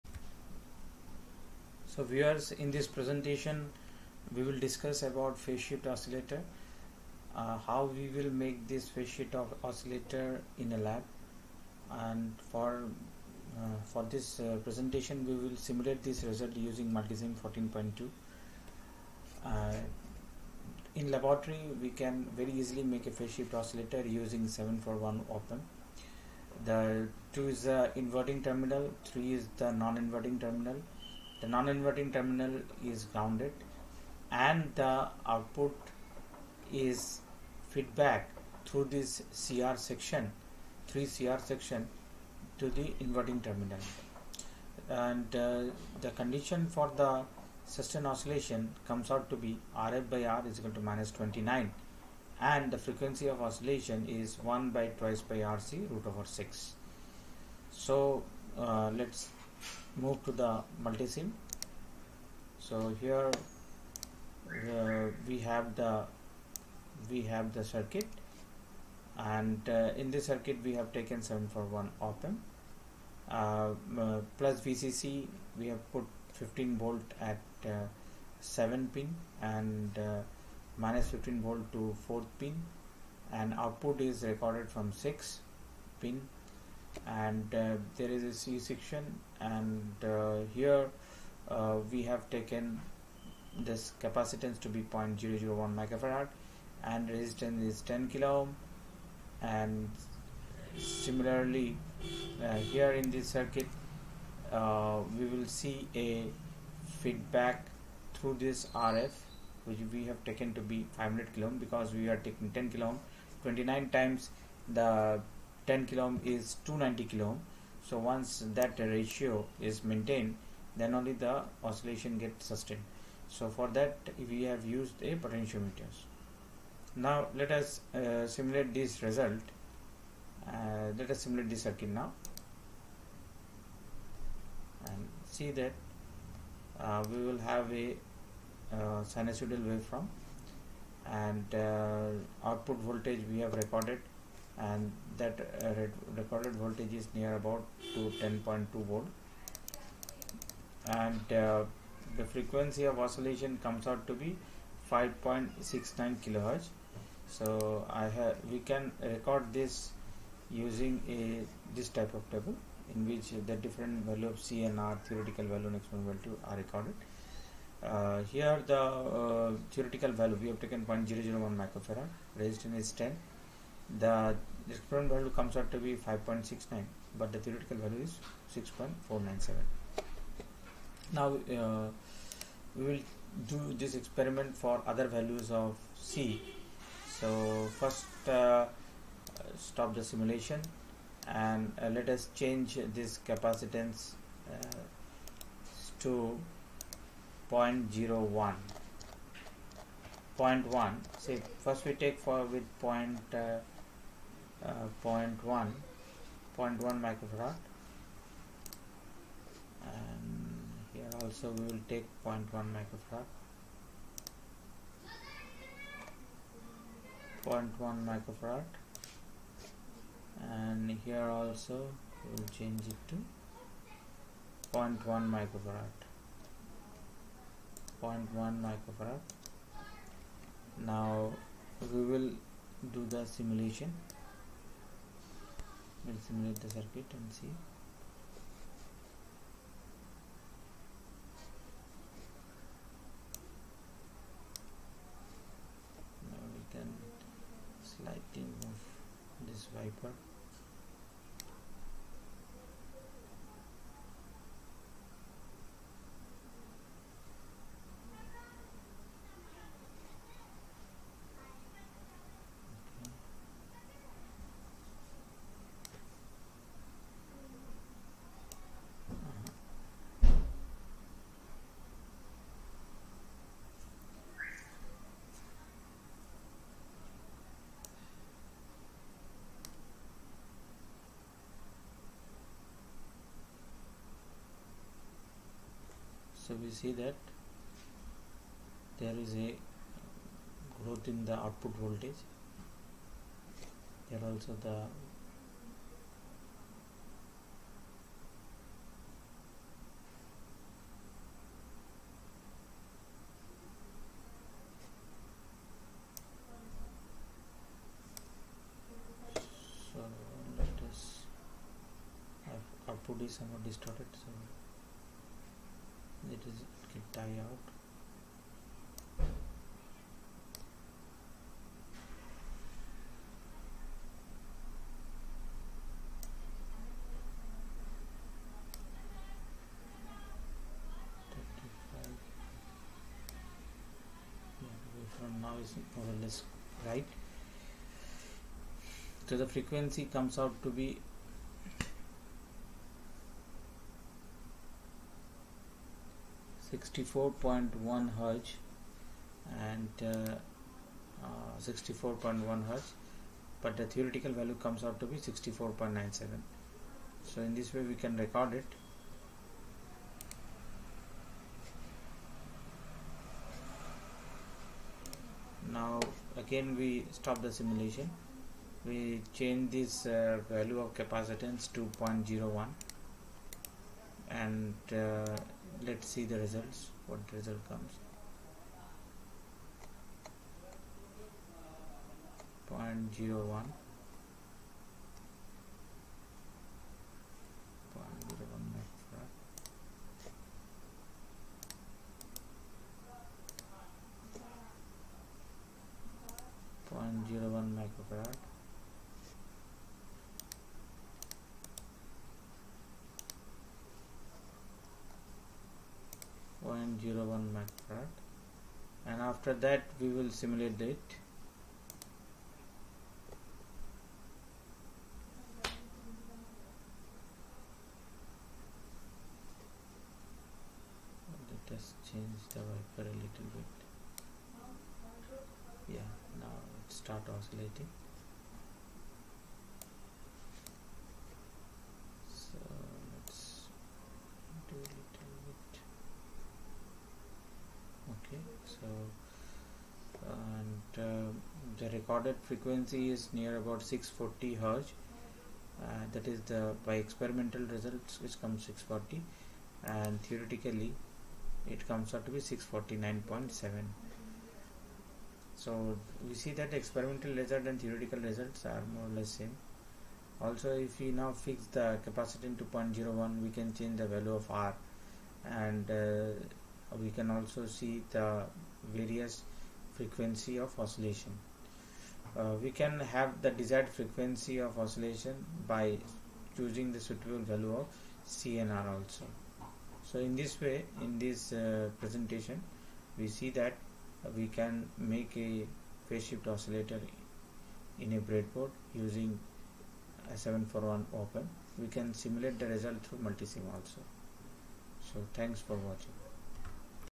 phase shift oscillator simulation using multisim 14.2
Phase shift oscillator is an audio frequency oscillator which incoperate positive feedback. The circuit can be simulated using multisim 14.2 and the frequency of oscillation can be recorded.